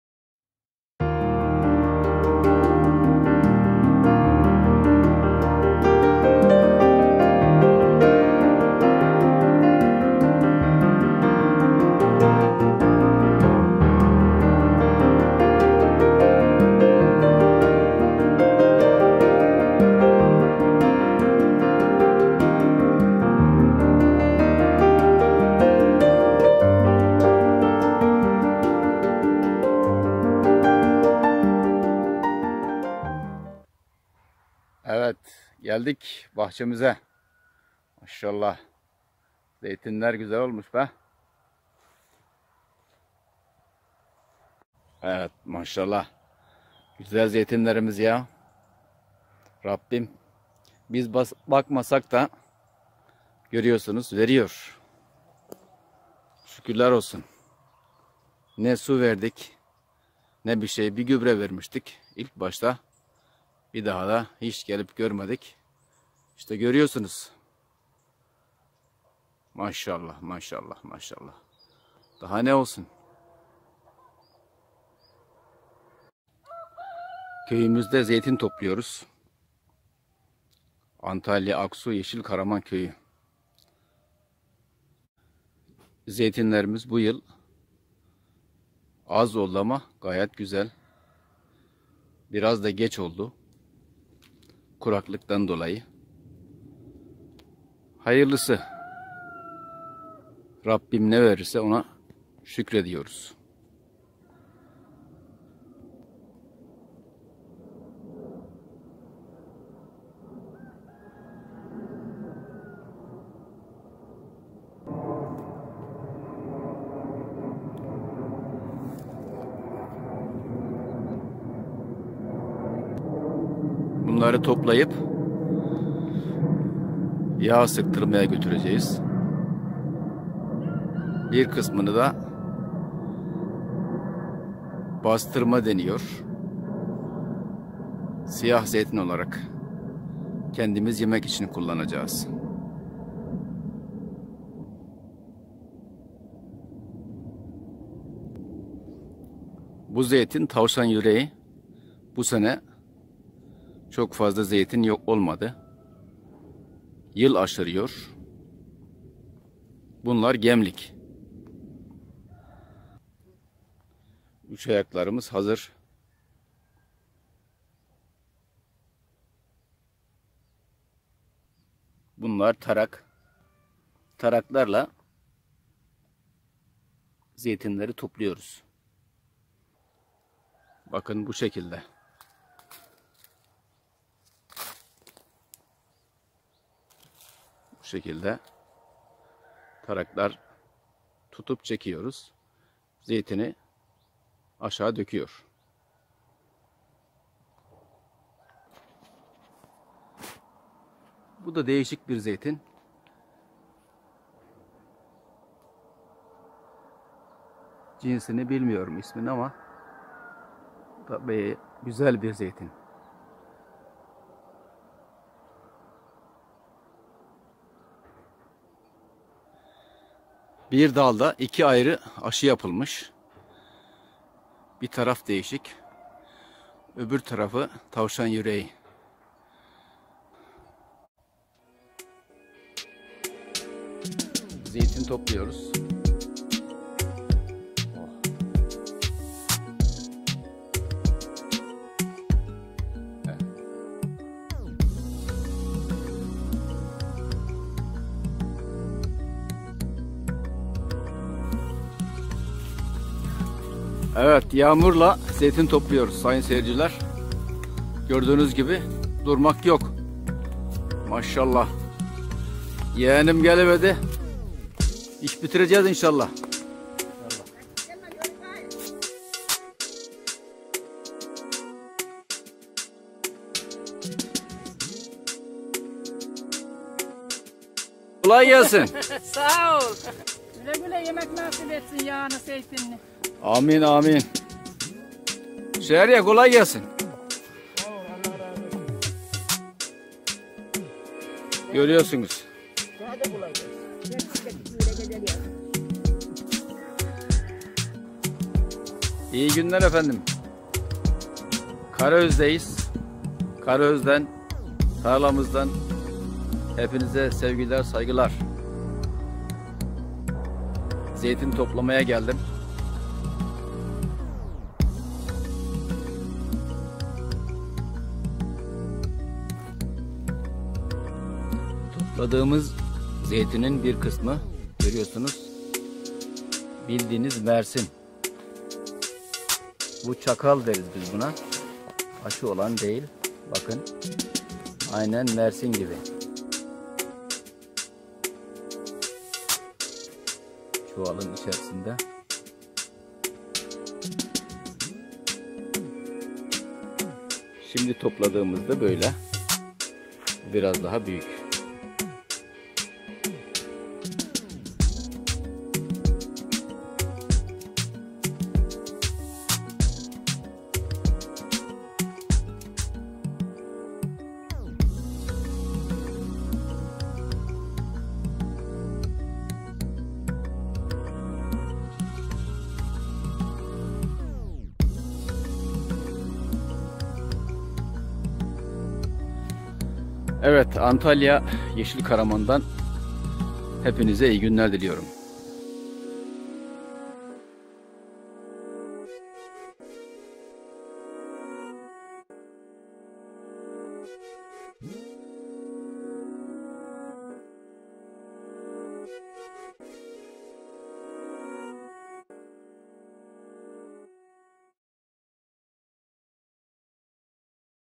Köyümüzde El ile Zeytin Toplama, Yeşilkaraman ve Karaöz, Aksu, Antalya
Video Çekim Tarihi: 3-16 Aralık 2020
Köyümüzde zeytin toplama işimiz, 3 Aralık 2020 Perşembe günü başlamış, havaların yağmurlu olması nedeniyle 16 Aralık 2020 Çarşamba günü sona ermiştir. Toplam 840 kg. zeytinimiz olmuştur. İyi seyirler dileriz.
Yeşilkaraman, Aksu, Antalya Türkiye